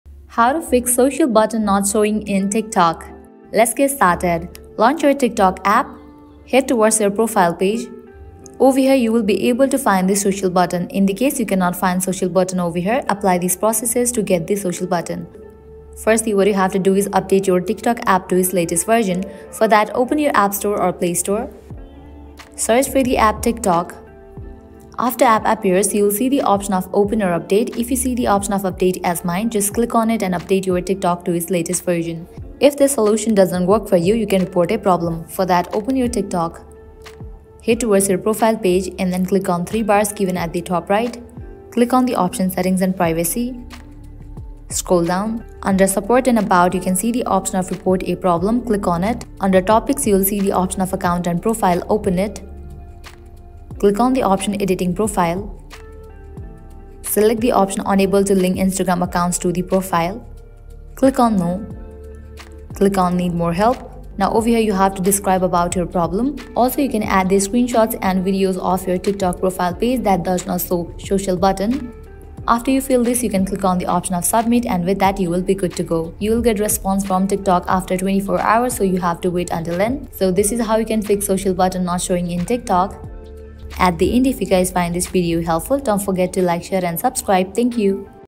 How To FIX SOCIAL BUTTON Not Showing In TikTok | Can’t Link Instagram and YouTube On TikTok
If you're facing issues with the social buttons not showing in TikTok or are unable to link your Instagram and YouTube accounts to your TikTok profile, follow these steps to troubleshoot and resolve the problem:

Timestamps:
0:00 Introduction
0:05 How To FIX SOCIAL BUTTON Not Showing In TikTok | Can’t Link Instagram and YouTube On TikTok

Ensure your TikTok profile is connected to your other social media accounts by following this guide to fix the social buttons and enable linking with Instagram and YouTube.